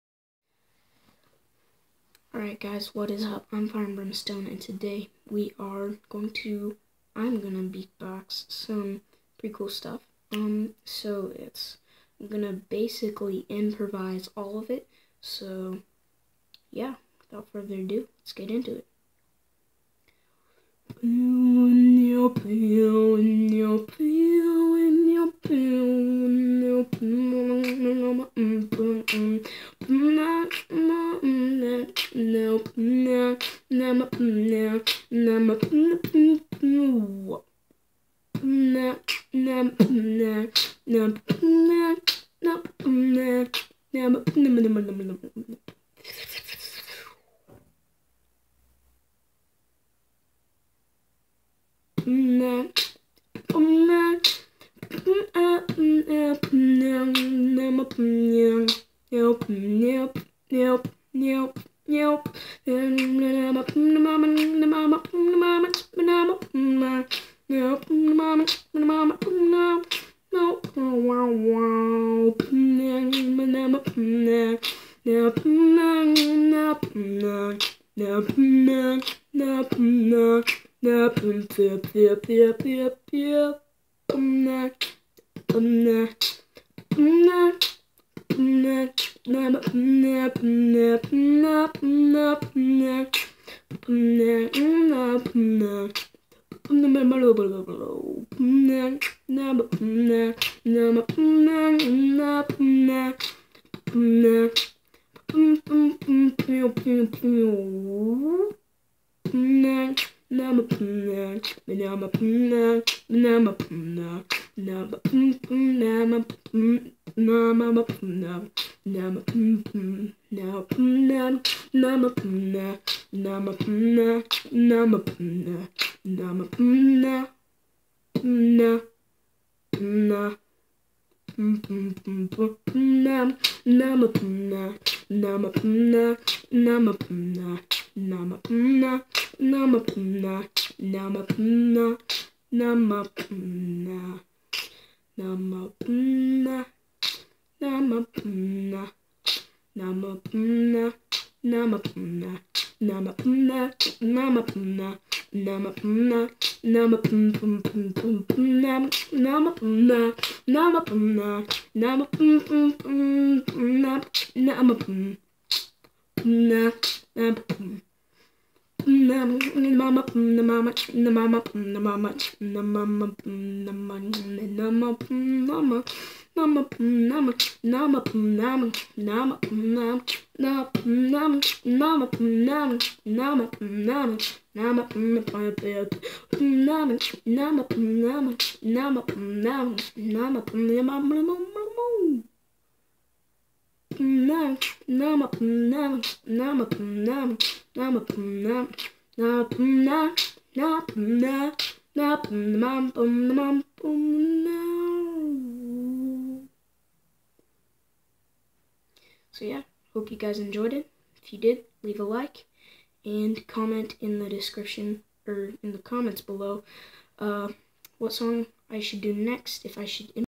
Beat boxing improvisations
Sorry the end got cut off, my phone died!
Let me know what song I should beat box next.